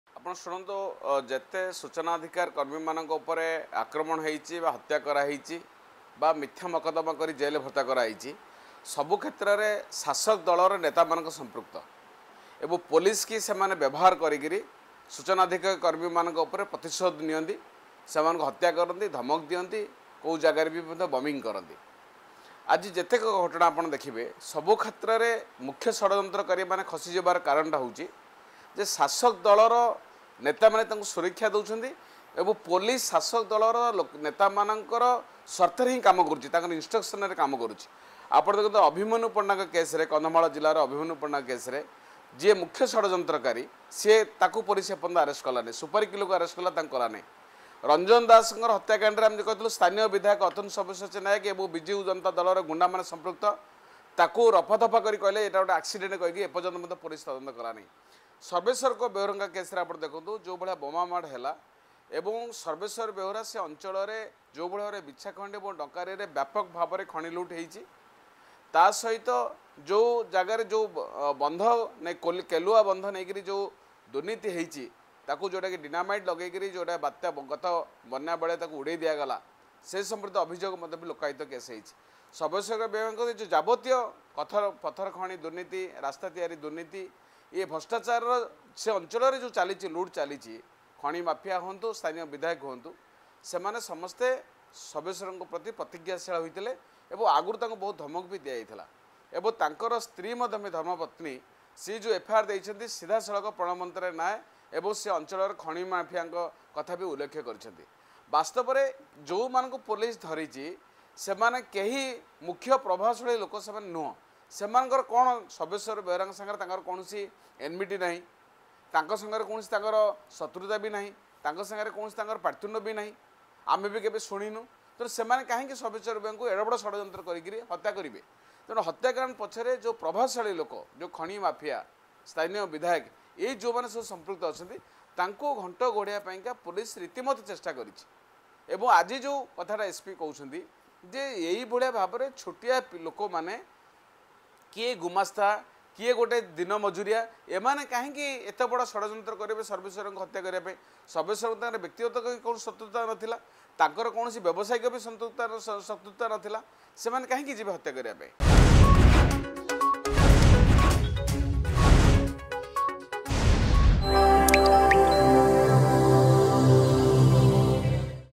By the help of police, the leaders of the ruling party are taking revenge on the RTI workers
ପୋଲିସର ବ୍ୟବହାର କରି ଶାସକ ଦଳର ନେତାମାନେ RTI କର୍ମୀଙ୍କ ଉପରେ ପ୍ରତିଶୋଧ ନିଅନ୍ତି ...
ଶୁଣନ୍ତୁ ସୂଚନା ଅଧିକାର କର୍ମୀ ପ୍ରଦୀପ ପ୍ରଧାନଙ୍କ ପ୍ରତିକ୍ରିୟା ...